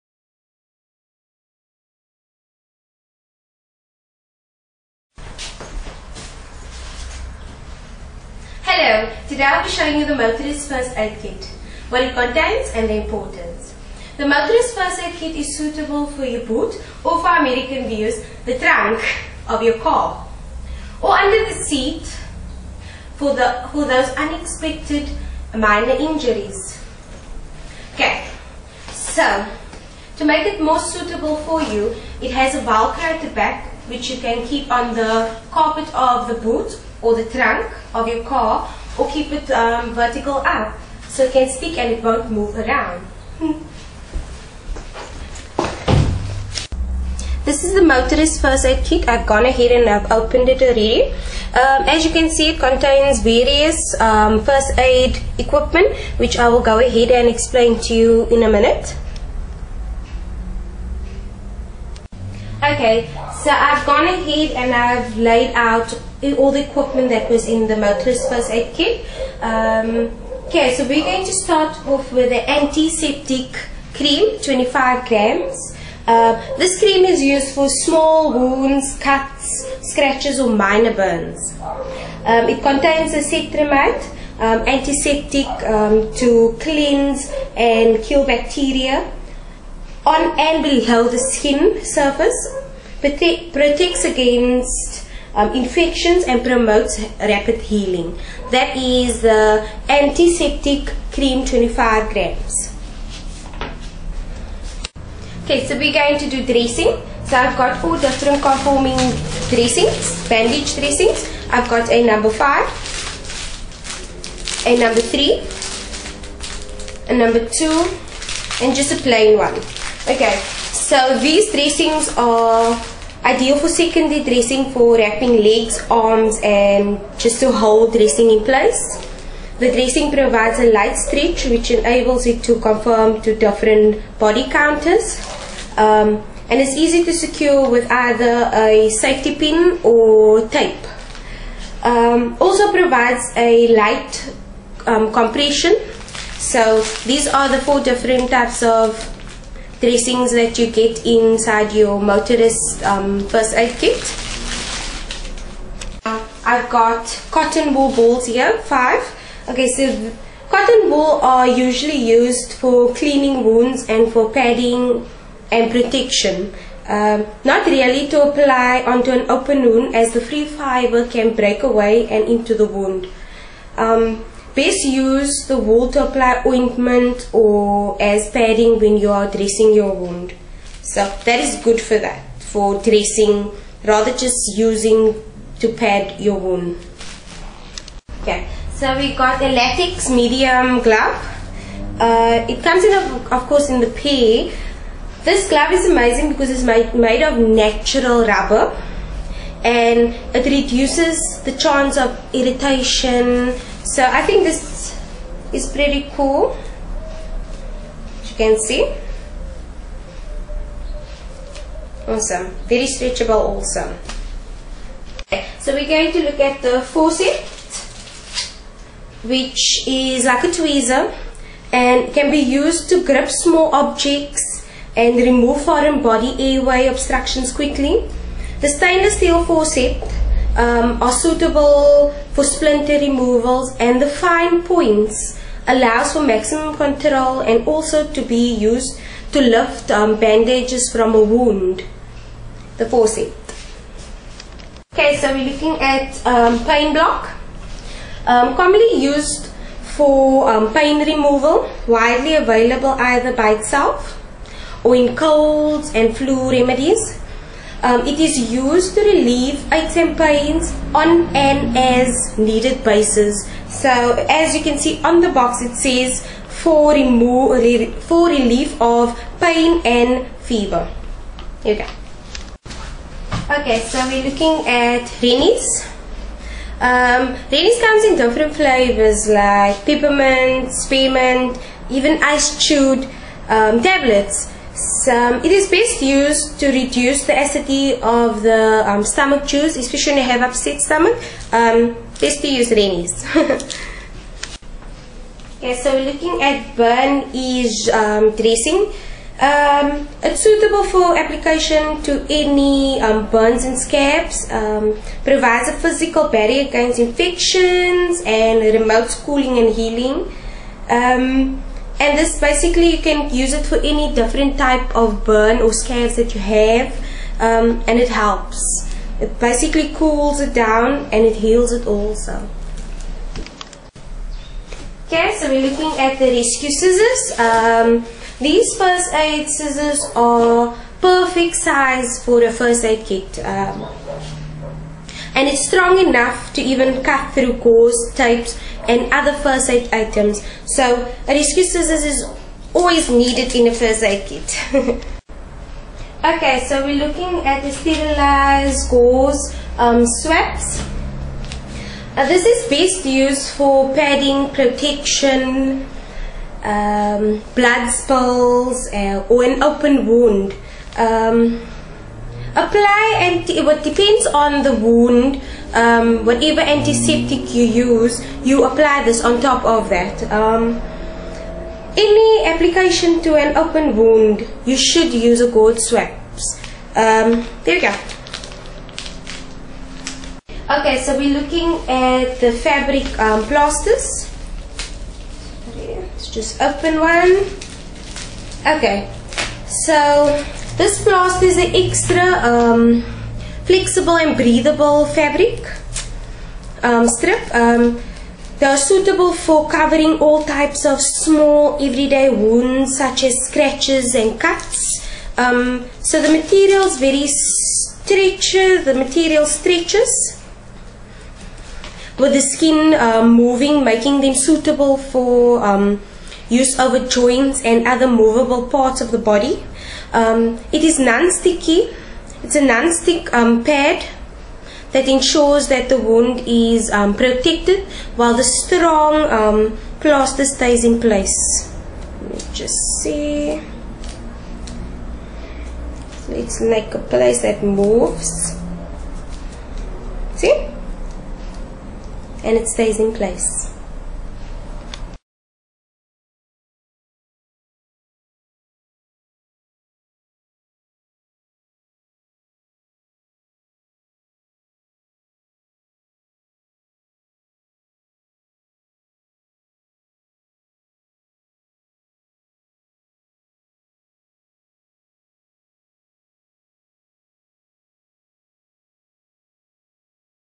Motorist First Aid Kit 310 Tactical Balance SA Part 1
The Motorist First Aid Kit 310 is suitable to have in your vehicle for those unexpected minor injuries on the road.